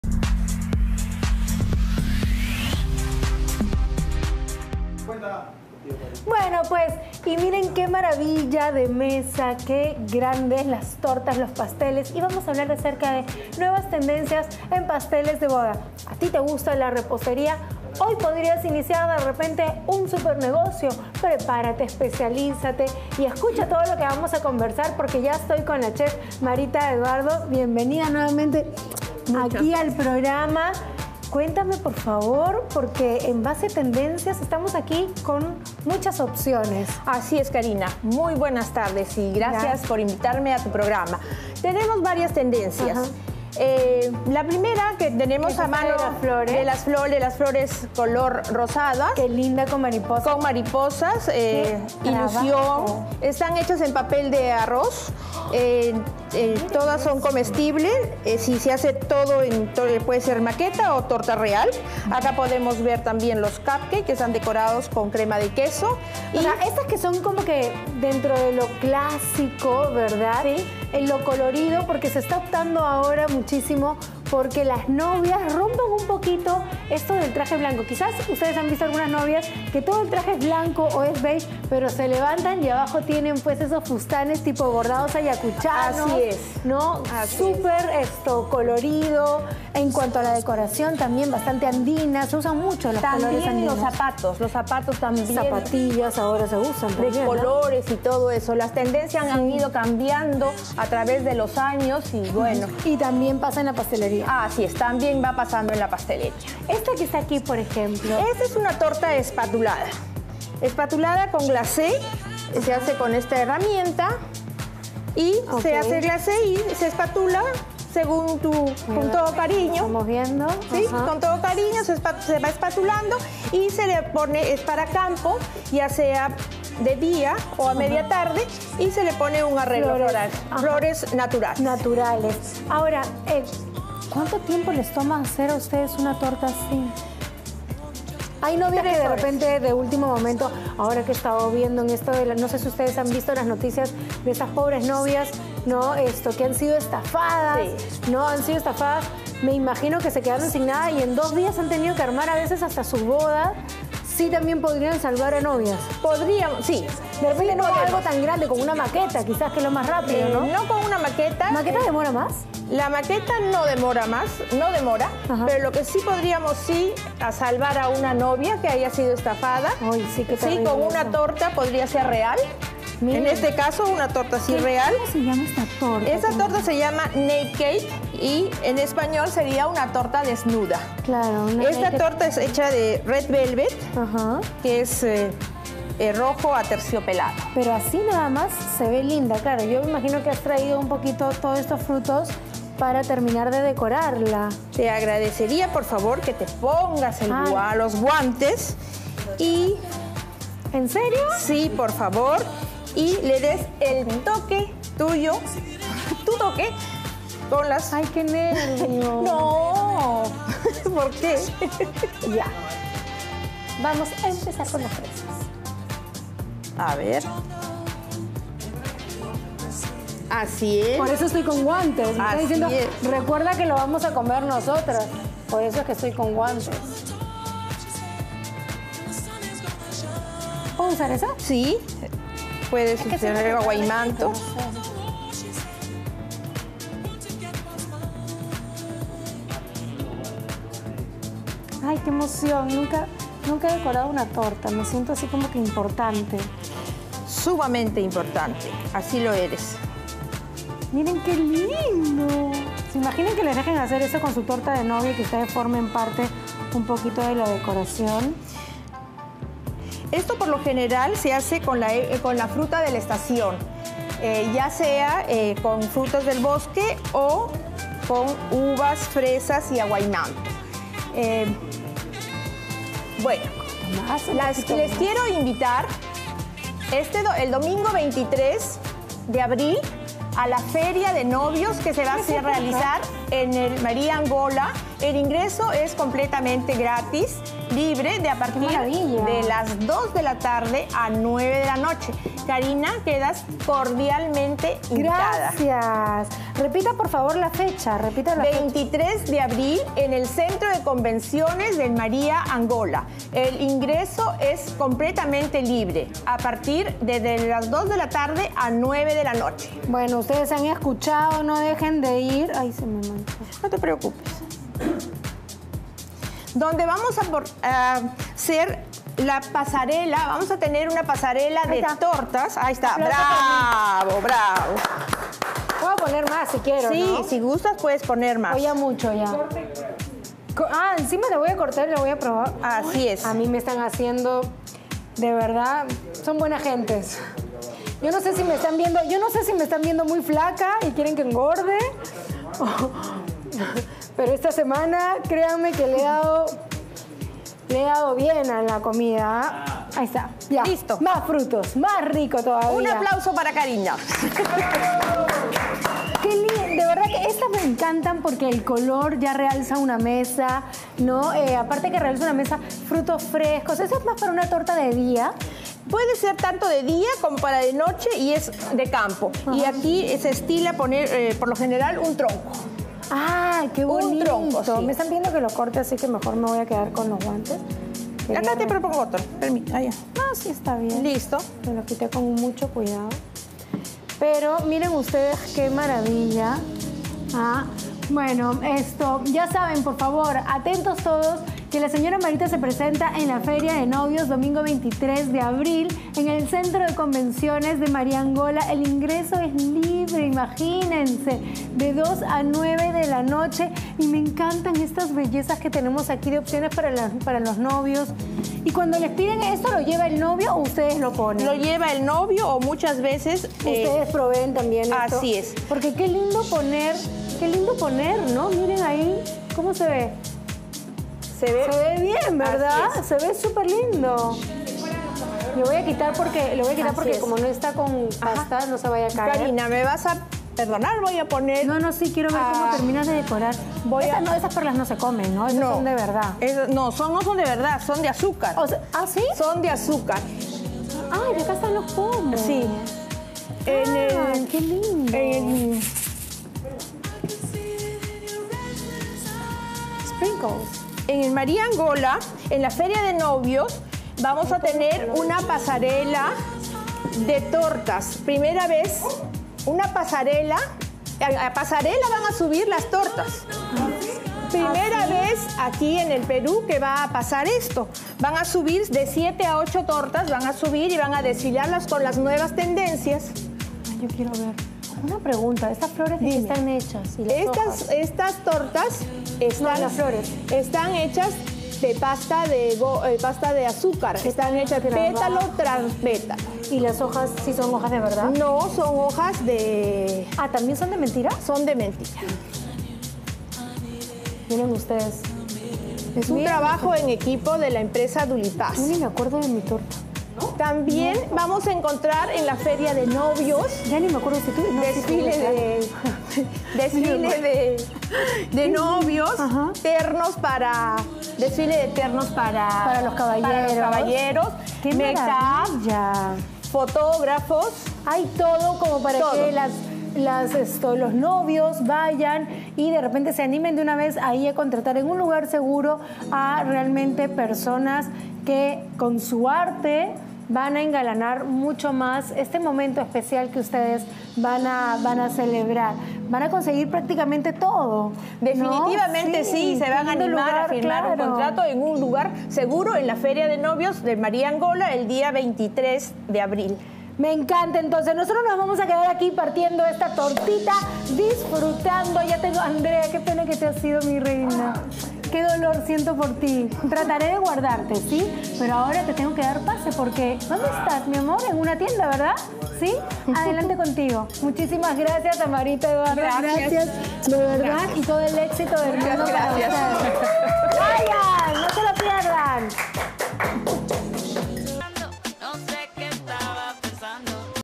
CAMPAÑA FERIA DE NOVIOS 2017 - I
Entrevista a Marita Eduardo - Chef Patissier de DUL&PAS. Se mostró nuevas tendencias en pasteles para bodas y se invito a los televidentes a la FERIA DE NOVIOS.